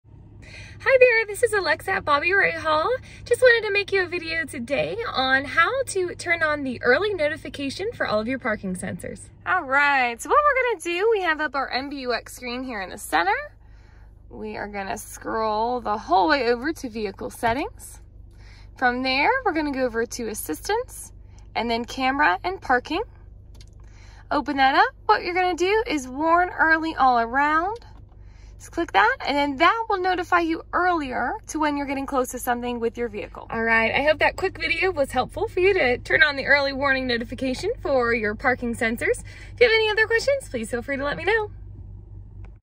Turn on Early Warning Notification for Parking Sensors 2021 Mercedes Benz
Turn on the earlier warning notification for your Mercedes Benz Parking Sensors.

For vehicles with MBUX!

Comment with more questions!